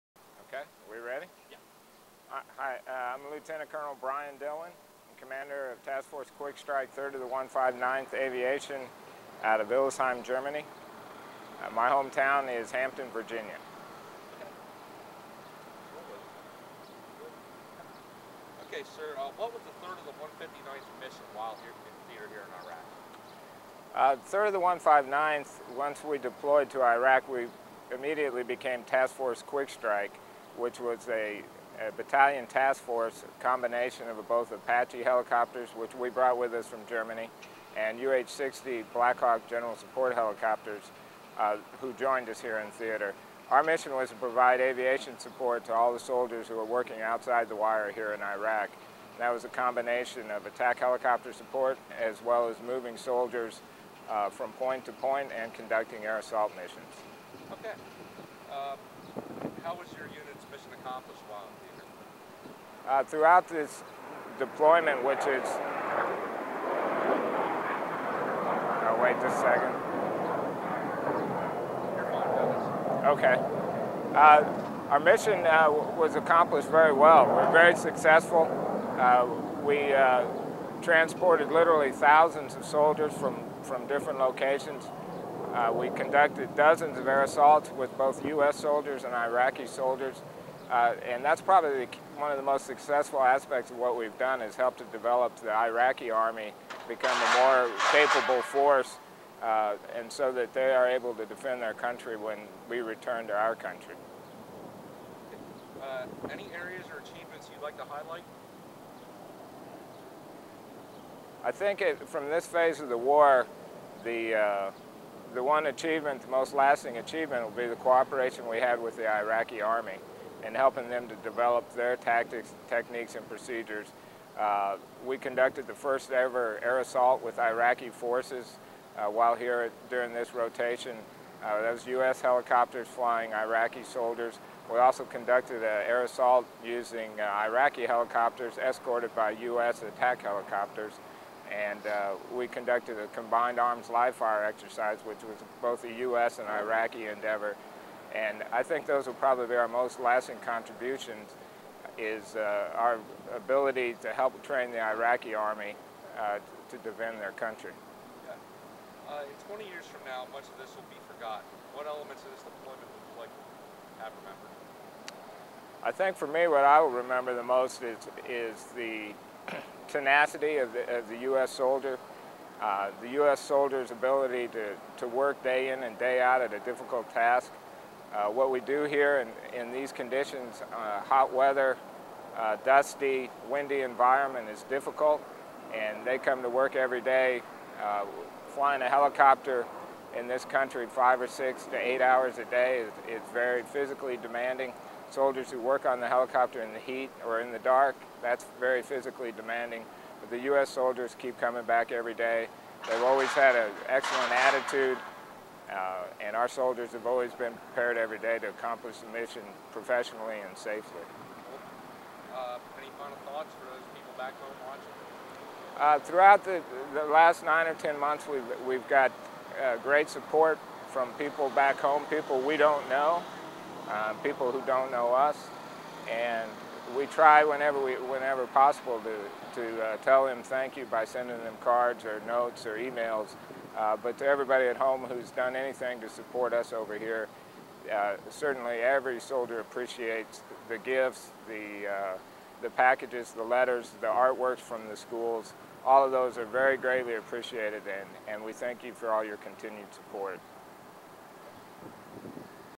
Lt. Col. Dillon
Lt. Col. Brian Dillon talks to a military reporter about the mission of the aviation brigade he commands in Iraq, the accomplishments of the unit during their deployment, helping develop the skills of Iraqi pilots, what his long-term memories of this experience will be for him and thanks those at home who have given them support during their deployment.

Interviewees:
• Lt. Col. Brian Dillon (Hampton, VA, US), Commander, Task Force Quick Strike, 3-159th Aviation

TRT: 4:13